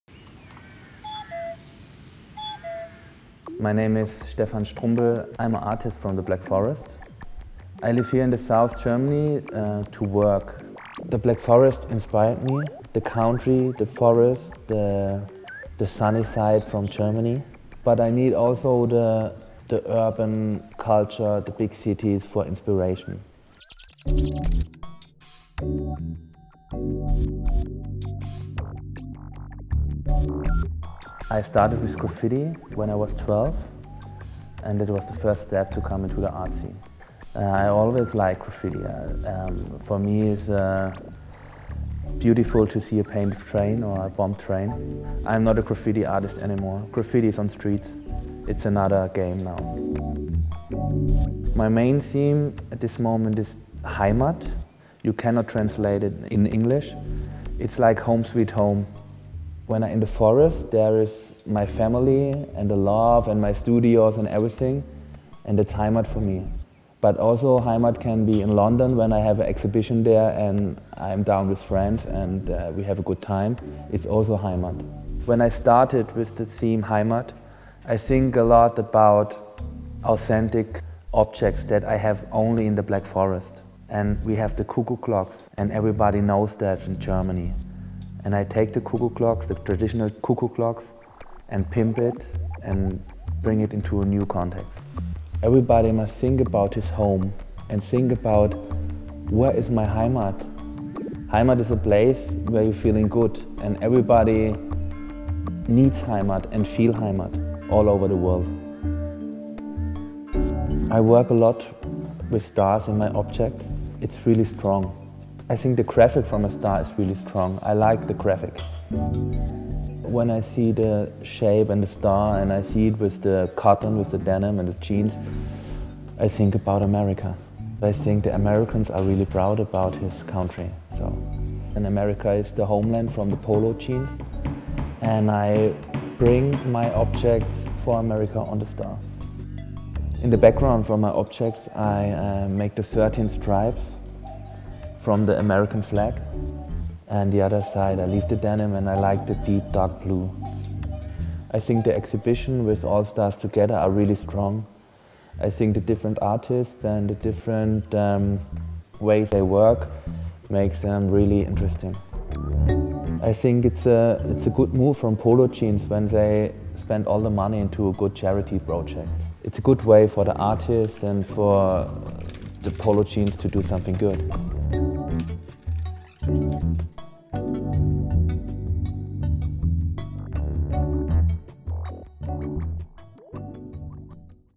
Stefan Strumbel - Polo Art Stars
Art Star No.16. HOME SWEET HOME, 2010. Mixed media and carved wood on denim.

Stefan Strumbel's Technicolor clocks and paintings focus on the concept of 'heimat', roughly translated as homeland or belonging. From his studio in the Black Forest, he uses images and objects strongly linked to cultural identity - such as cuckoo clocks and pictures of women in national costume - and mixes them with homegenised symbols that include shopping trolleys, guns and guitars, to question where exactly cultural identity is heading.